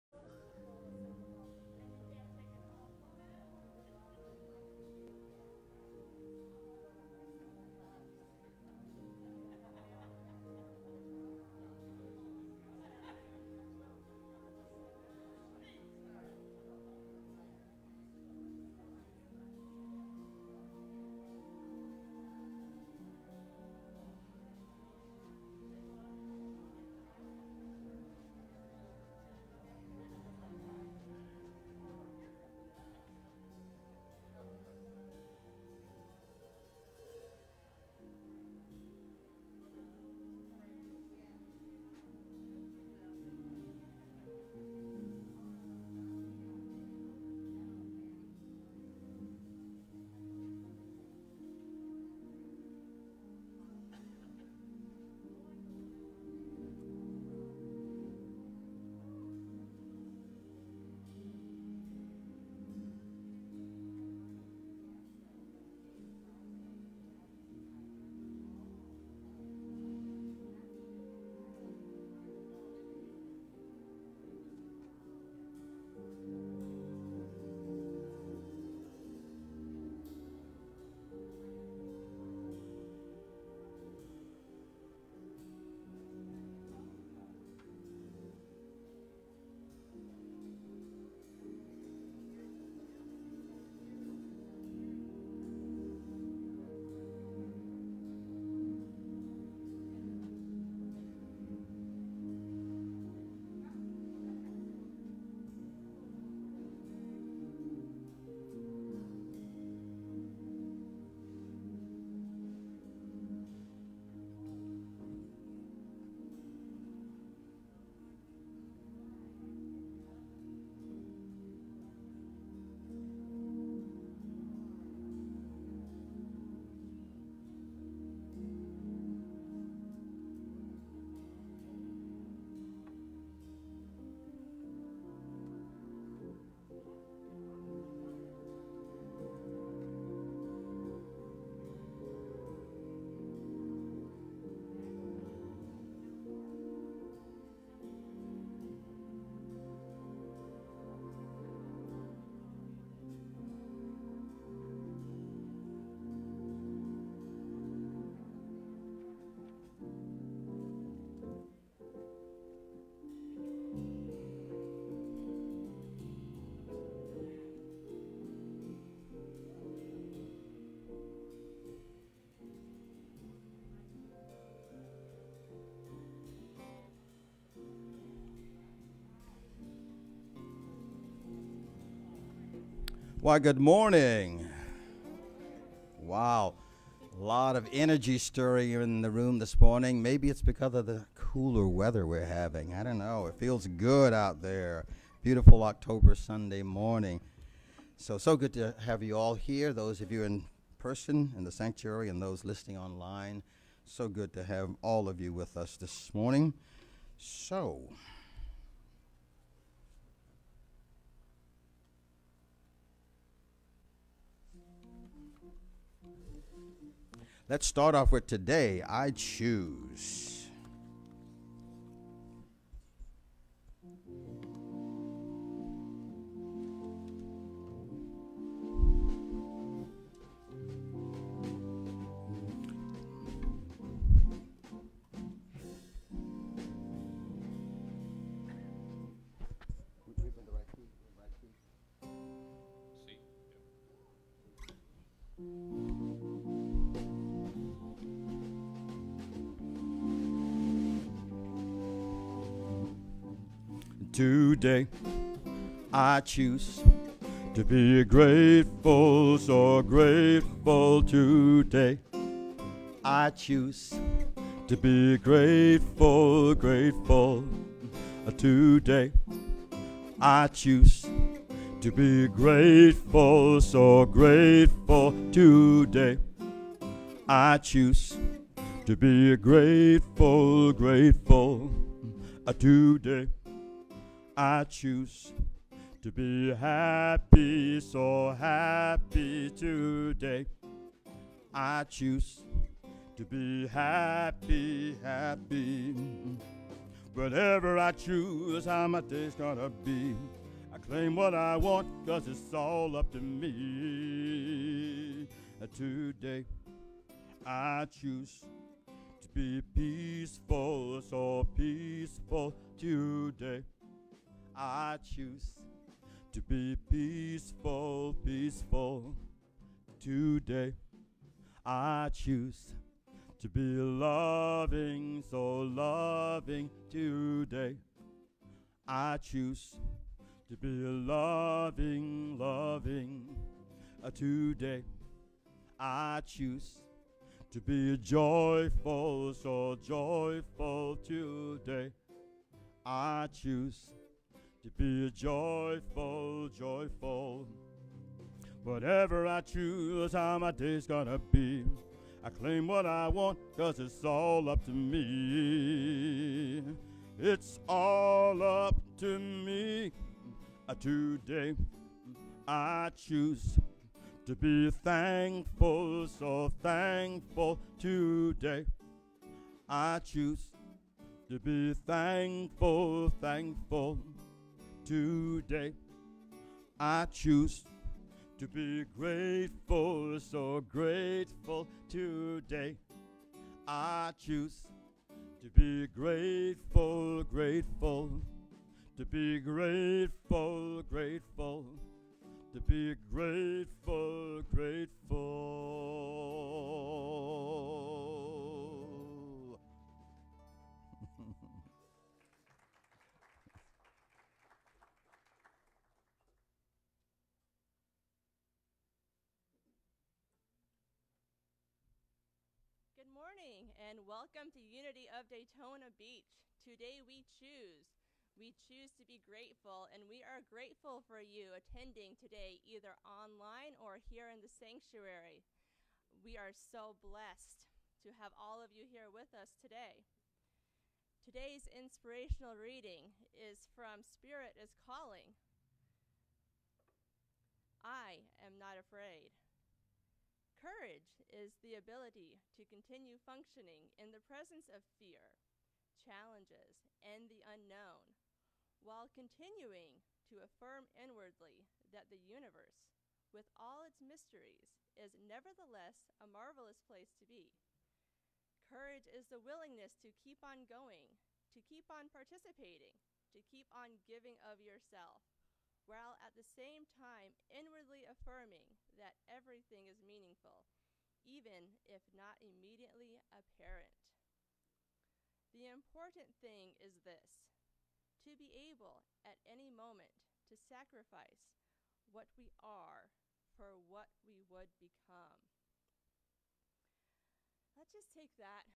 Sunday Service delivered by Rev. Teresa Curry, Oct. 17, 2021
Part 1 of the Sunday Service